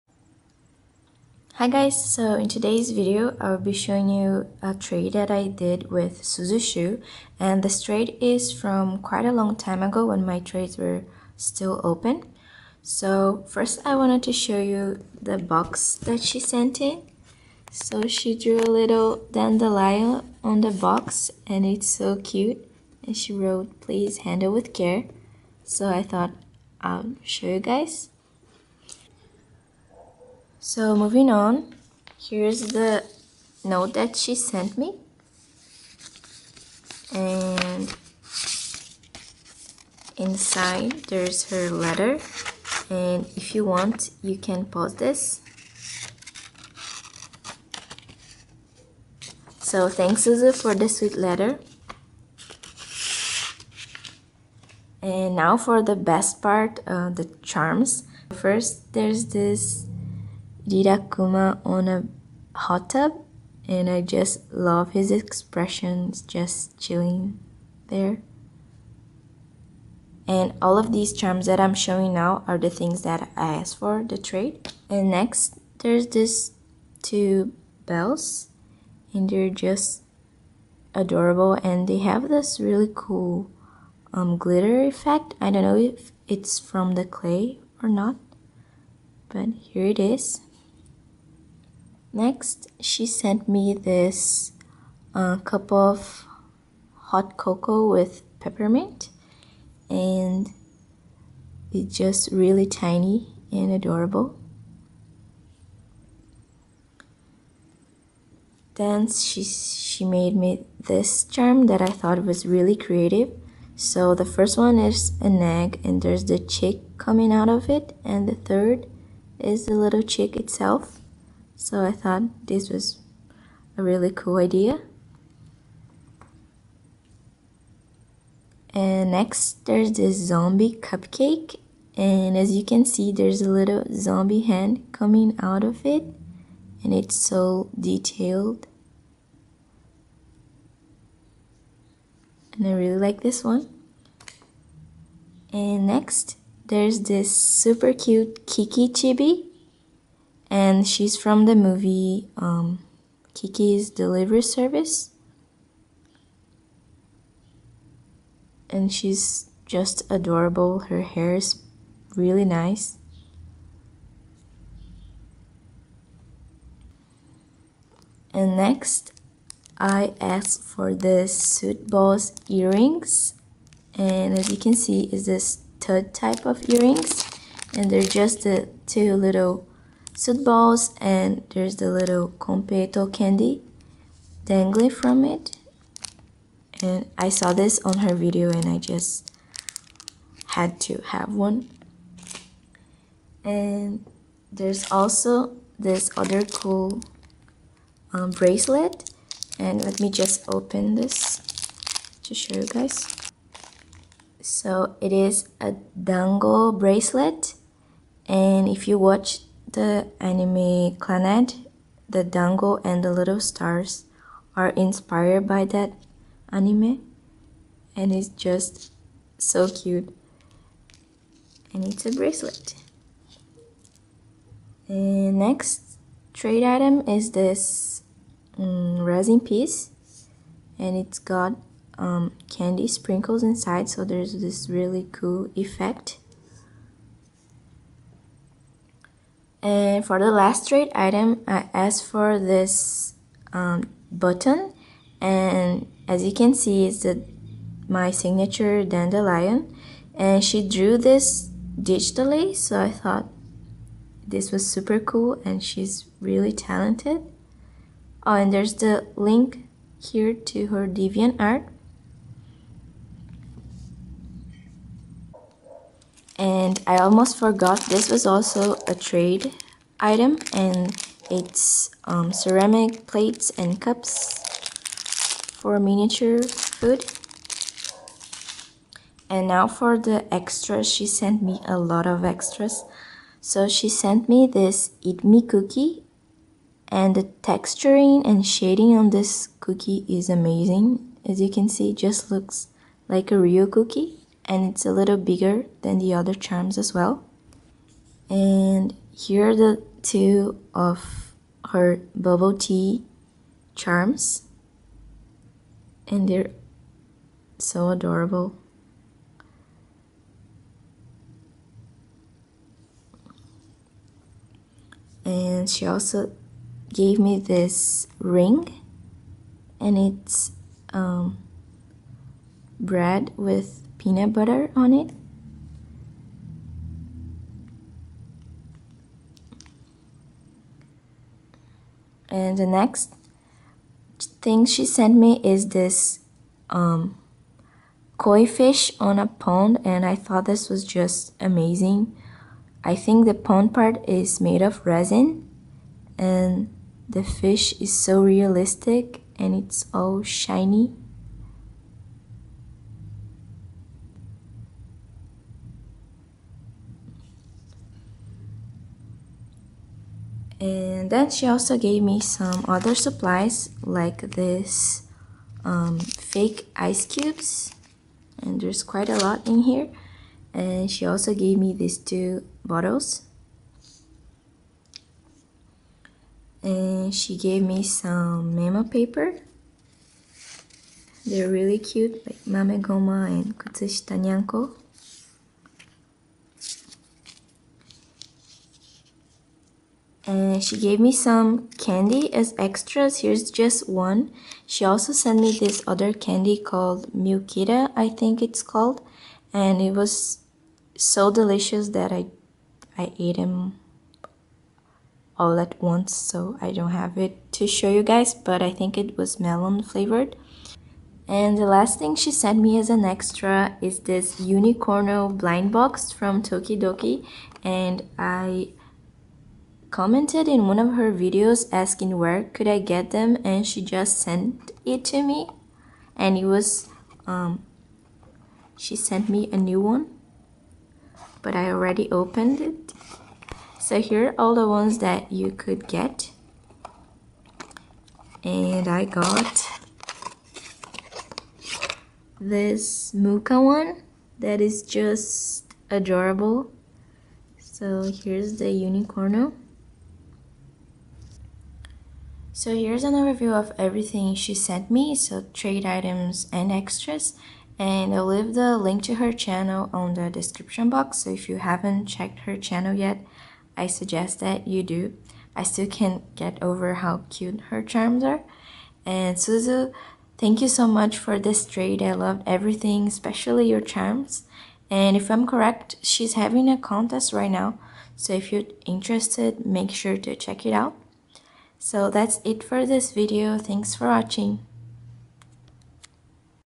Trade Package from Suzushoe!
Hi guys! I actually had to film this again because I lost the footage of the original opening video, (Sorry Suzu!).

Wanna send me notes/ gifts?

Stephanie Y.
Caixa Postal 19029
Curitiba - PR Brazil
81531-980